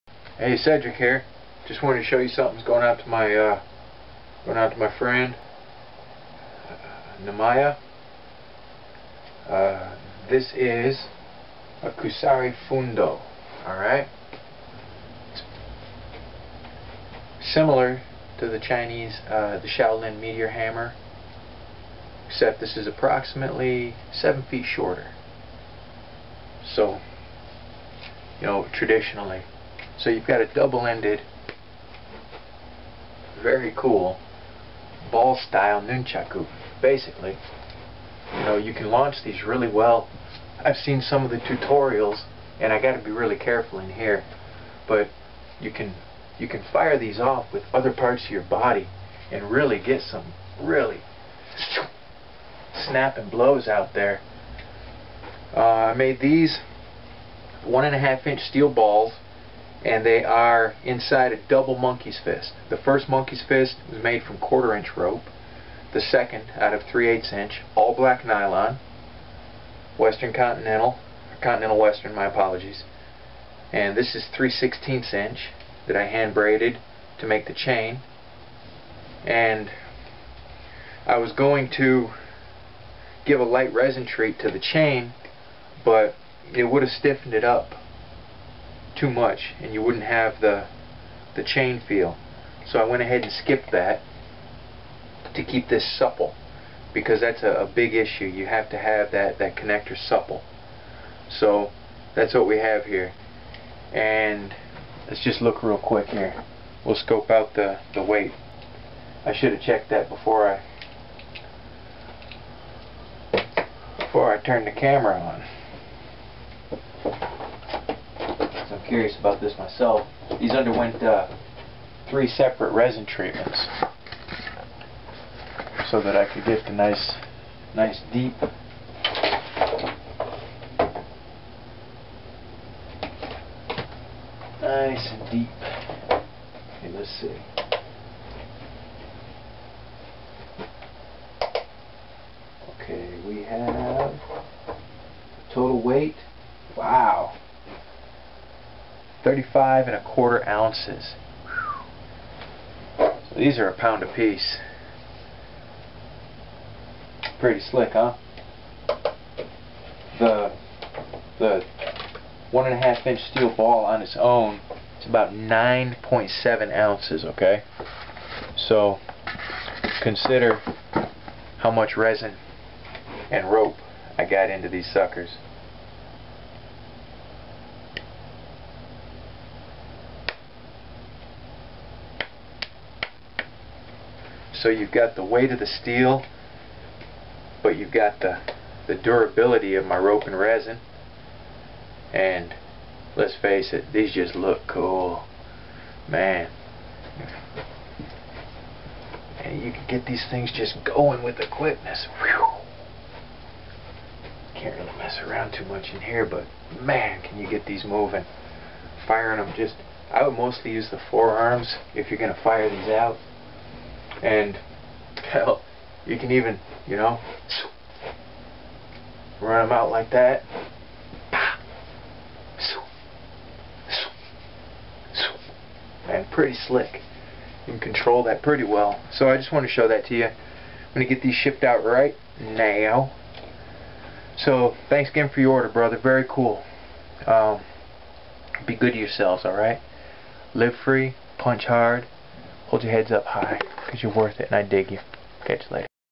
Kusari Fundo, Rope Rendition of the Traditional Japanese Weapon
This Kusari-Fundo was a special order, requested by a friend of mine. The Kusari Fundo is a traditional Japanese weapon in the flail spectrum of weaponry.